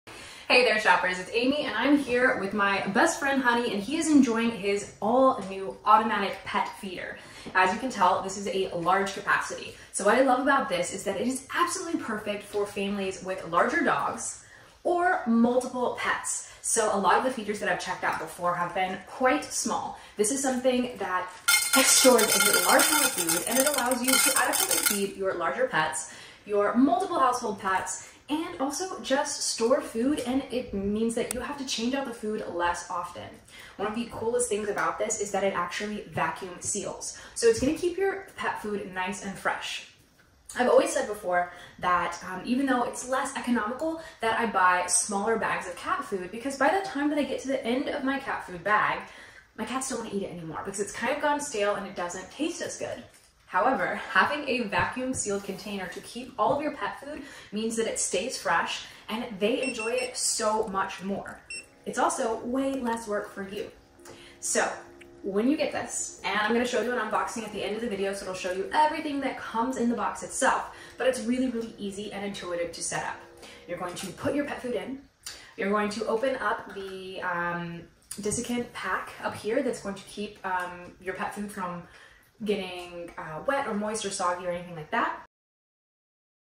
Dokoo Large Pet Feeder - Making Mealtime a Breeze!
@shoppingwithamy Thanks for sharing:)
15L Large Capacity:Can sustain a large dog for up to 15 days, alleviating the daily hassle of refilling and providing a reliable solution for pet owners with busy schedules.
✨Vacuum Sealed:Ensures that each serving maintains its freshness, offering a delectable and wholesome meal for your dog every time.